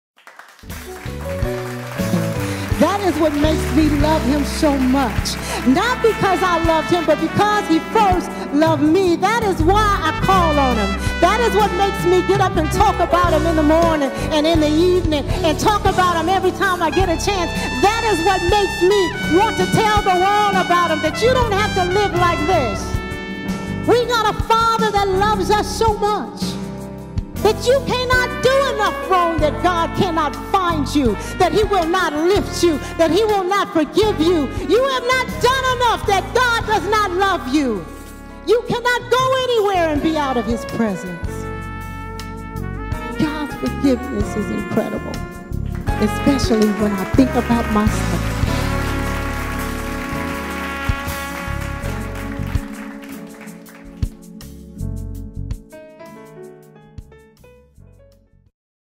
FATHER FORGIVE ME - MljacksonMinistries.com
sermon snippet by Minister Michelle L Jackson. complete sermon available